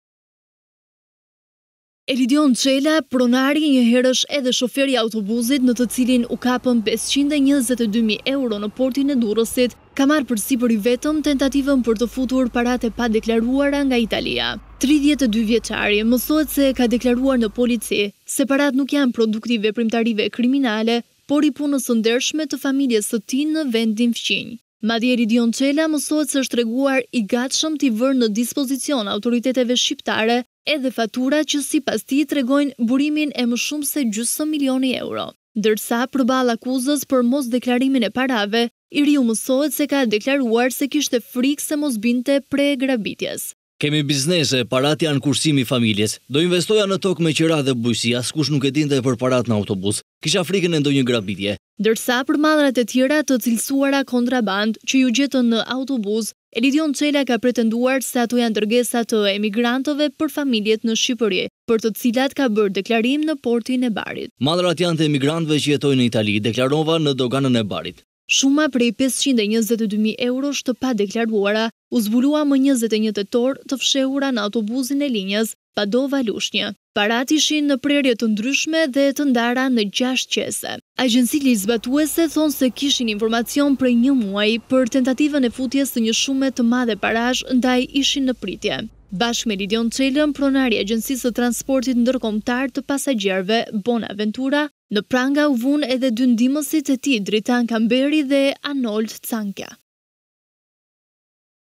522 mijë € të padeklaruara në autobus, Çela: Janë kursime të familjes, do t'i investoja në bujqësi
32 vjeçari Elidon Çela i arrestuar në Portin e Durrësit me mbi gjysmë milioni euro, ka deklaruar se paratë nuk vijnë nga veprimtari kriminale, por nga kursimet e familjes me biznese në Itali. Madje mësohet se ai është treguar i gatshëm ti vënë në dispozicion autoriteteve shqiptare edhe faturat që sipas tij tregojnë burimin e parave që tentoi ti fusë në Shqipëri në shkelje të ligjit.  Elidon Çela, pronari njëherësh shoferi i autobusit në të cilin u kapën 522 mijë euro në portin e Durrësit, me 21 tetor 2022 ka marrë përsipër i vetëm tentativën për të futur paratë e padeklaruara nga Italia.

32 vjeçari mësohet se ka deklaruar në polici se paratë nuk janë produkt i veprimtarive kriminale, por i punës së ndershme të familjes së tij në vendin fqinj. Madje Elidon Çela mësohet se është treguar i gatshëm ti vendosë në dispozicion autoriteteve shqiptare edhe faturat që sipas tij tregojnë burimin e më shumë se gjysmë milioni euro. Ndërsa përballë akuzës për mosdeklarimin e parave, i riu mësohet se ka deklaruar se kishte frikë se mos binte pre e grabitjes.

“Kemi biznes, paratë janë kursime të familjes, do investoja në toka me qira dhe bujqësi. Askush nuk e dinte për paratë në autobus, kisha frikën e ndonjë grabitjeje,” tha ai.

Ndërsa për mallrat e tjera të cilësuara kontrabandë që iu gjetën në Autobus, Elidon Çela ka pretenduar se ato janë dërgesa të emigrantëve për familjet në Shqipëri, për të cilat ka bërë deklarim në Portin e Barit.

“Mallrat janë të emigrantëve që jetojnë në Itali, i deklarova në doganën e Barit,” tha ai.

Shuma prej 522 mijë eurosh e pa deklaruar, u zbulua më 21 tetor të fshehura në autobusin e Linjes Padova-Lushnje.

Paratë ishin në prerje të ndryshme dhe të ndara në 6 qese.

Agjensitë ligjzbatuese thonë se kishin informacion prej një muaji për tentativën e futjes së një shumë të madhe parash ndaj ishin në pritje.

Bashkë me Elidon Çelën, pronar i agjensisë së transportit ndërkombëtar të pasagjerëve “Buon Aventura” në pranga u vunë edhe dy ndihmësit e tij Dritan Kamberi e Anold Cankja.